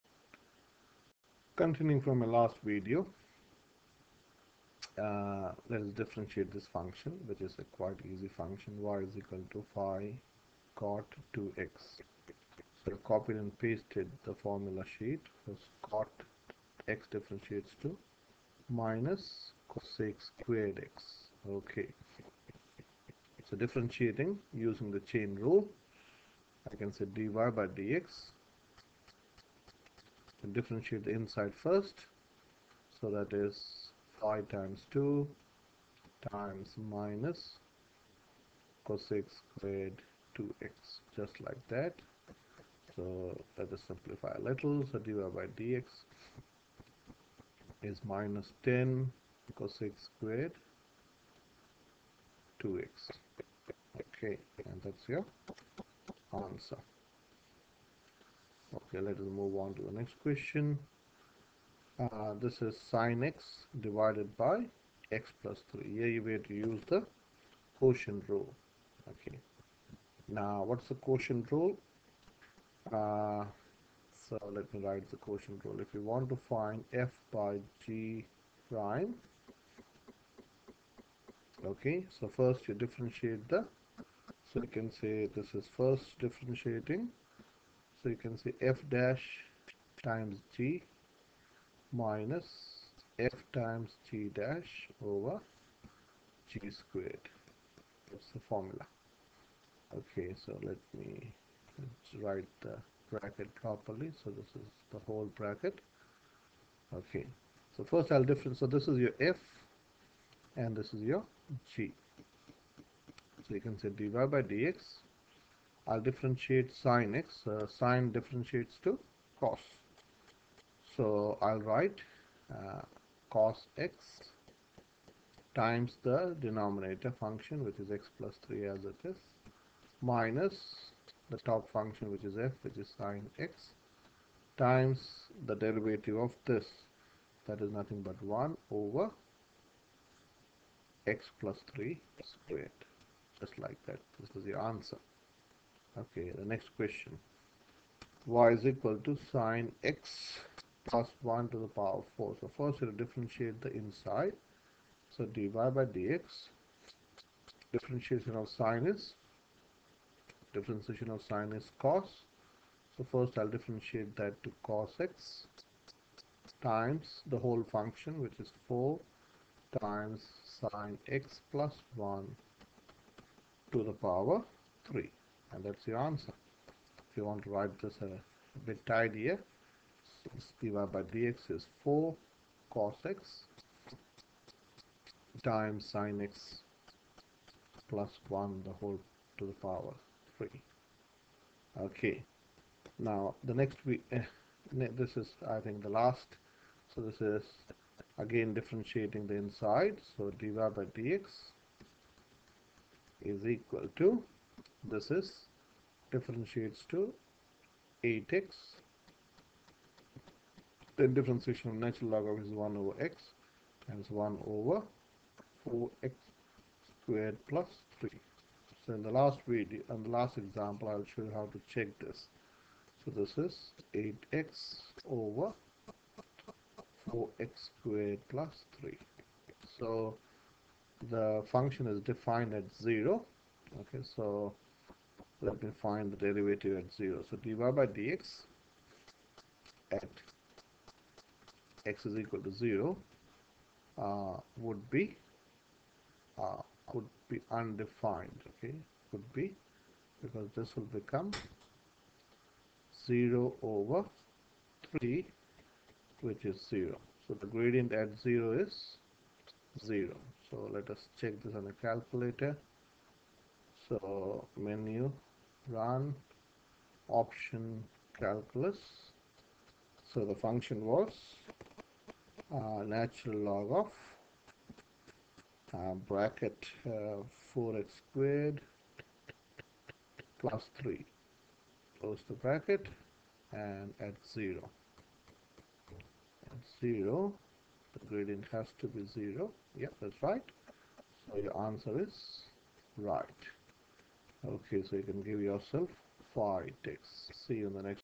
Derivative of (5Cot(2x)); (Sinx/(x+3)); (Sinx+1)^4 and ln(4x^2+3)
In this video I have shown how to differentiate using different rules of differentiation and checking your answer on a graphic calculator